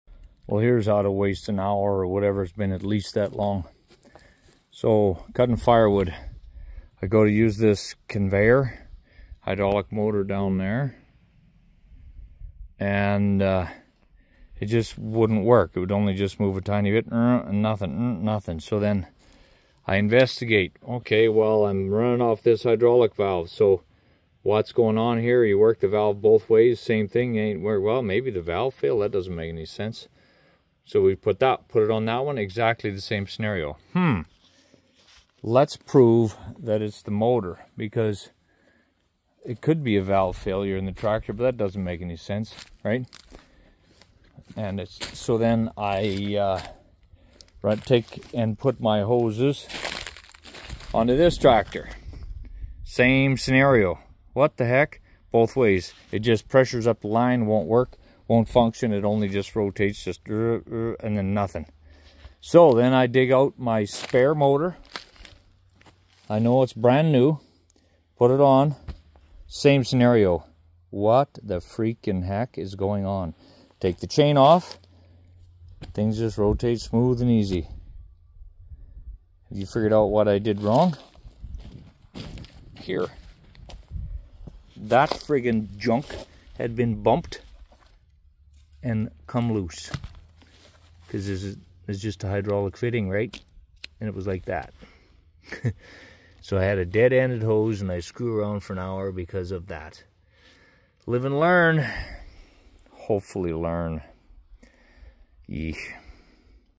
rookie mistake hydraulic problem diagnosis
rookie mistake hydraulic problem diagnosis. Here's a great example of jumping to conclusions instead of trying to prove your diagnosis is not correct and assuming you are right! As long as I get smarter ( I hope...)